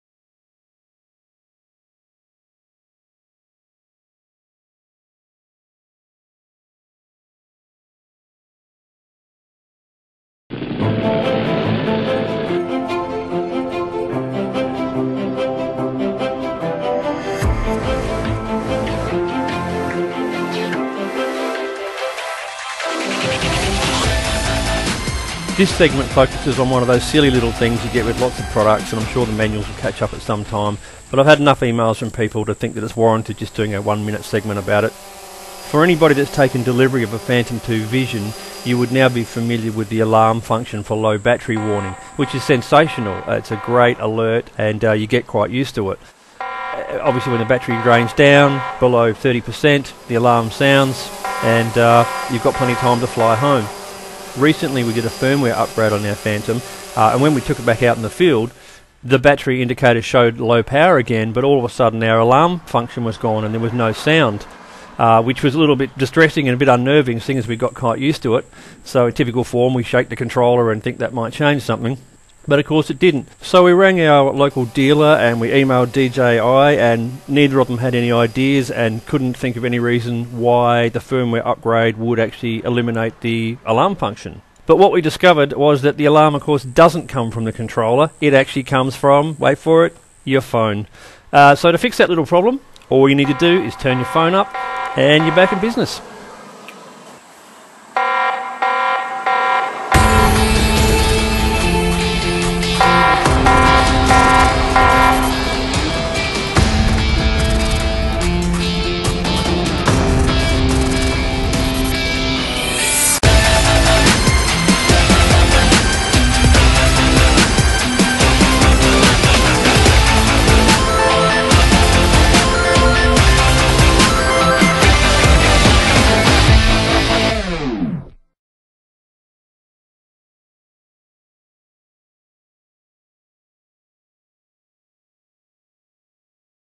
DJI Phantom 2 Quadcopter Phone App fail safe issue - BEWARE! (Tip #7)
Once you've downloaded your DI Phantom Phone app beware...
The Low battery fail safe alarm may not work on dji phantom 2 vision, no audio on battery alarm. Explained!

Its a dumb one! But seemed no-one could answer it!
Don't get caught out on a no warning phantom crash!!!

A simple checklist that we hope will minimise any chance of a "fly away" for you

DJI Phantom - DJI Iphone App
DJI Phantom - DJI smart phone App
DJI Phantom - DJI phone issues
DJI Phantom - Problems are firmware upgrade?

Filmed on a Panasonic Lumix Camera

for our list of upcoming tips